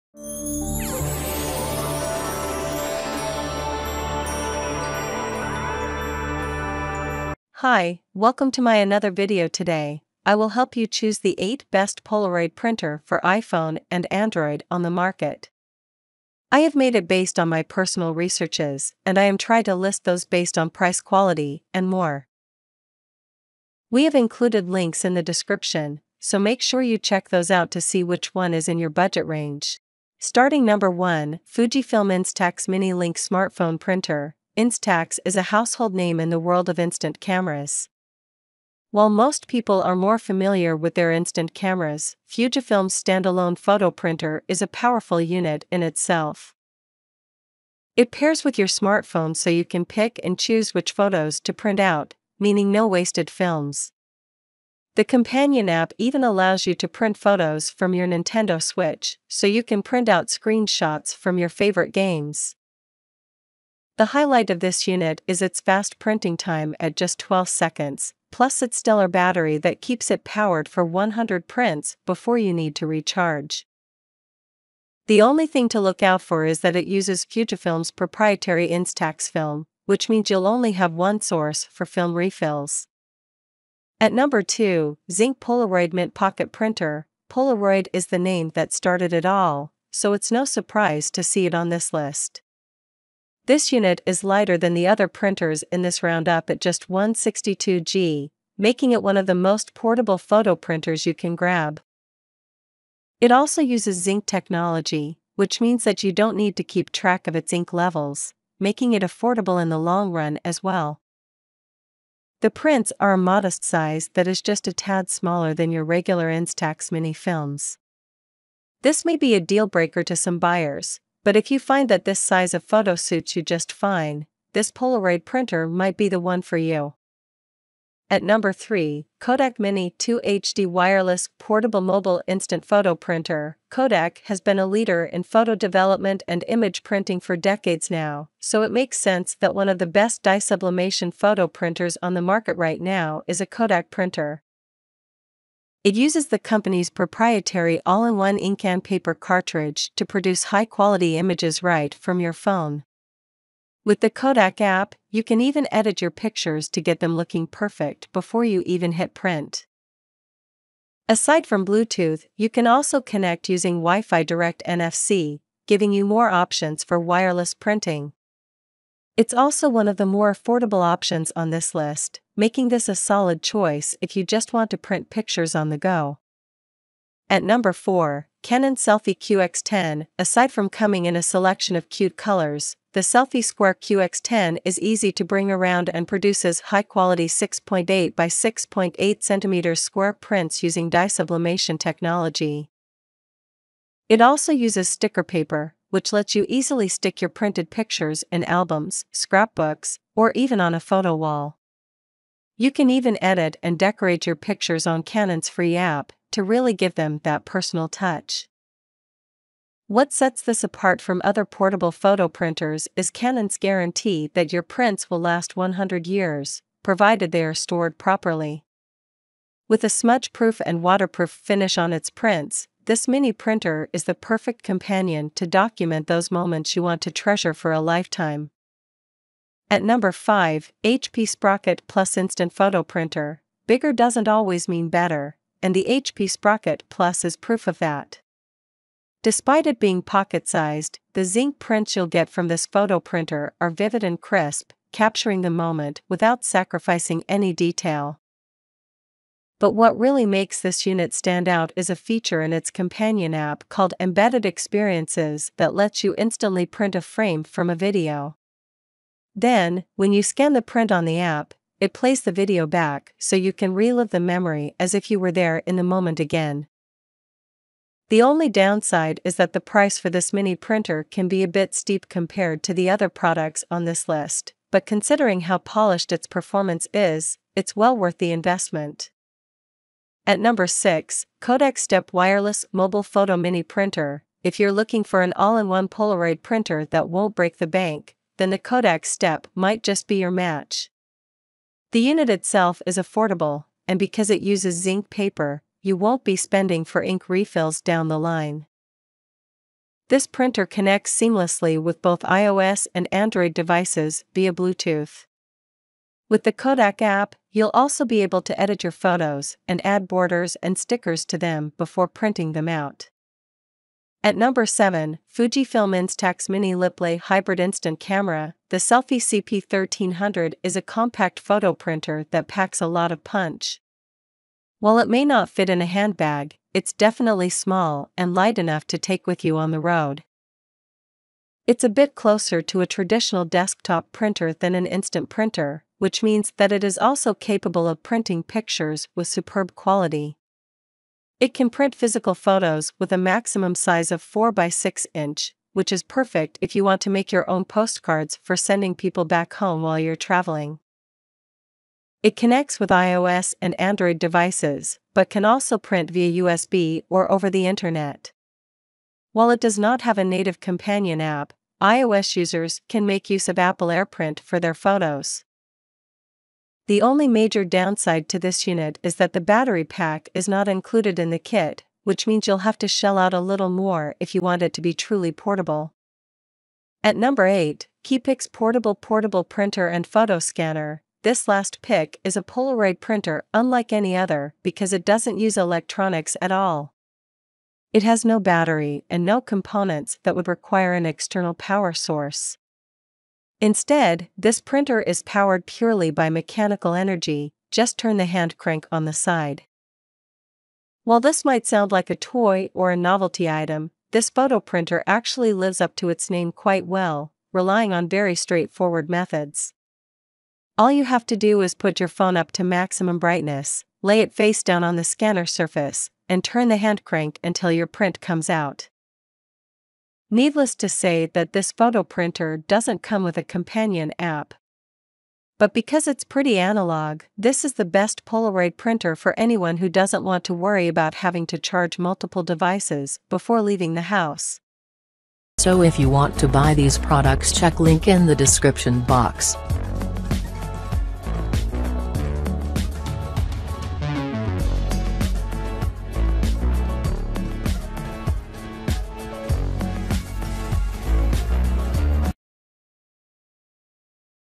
Best Polaroid Printer For iPhone & Android: Buying Guide
If you’re looking for the best Polaroid printer that can turn your smartphone into an instant camera, look no further! Click here to find out our top picks!

List of " Best Polaroid Printer For iPhone & Android"
👉1.Fujifilm Instax Mini Link Smartphone Printer
👉2.Zink Polaroid Mint Pocket Printer
👉 3.Kodak Mini 2 HD Wireless Portable Mobile Instant Photo Printer
👉4.Canon SELPHY QX10
👉5.HP Sprocket Plus Instant Photo Printer
👉6.KODAK Step Wireless Mobile Photo Mini Printer
👉7.Fujifilm Instax Mini Liplay Hybrid Instant Camera
👉8.KiiPix Portable Portable Printer & Photo Scanner

Moment cameras have been around since the 1940s, with names like Polaroid and Instax being inseparable from the little, enchanting prints they produce.

While the Polaroid printer fell in notoriety with the ascent of computerized simple to use cameras, moment cameras are getting back in the saddle with a cutting edge curve - the greater part of them currently cooperate with your cell phone.

Music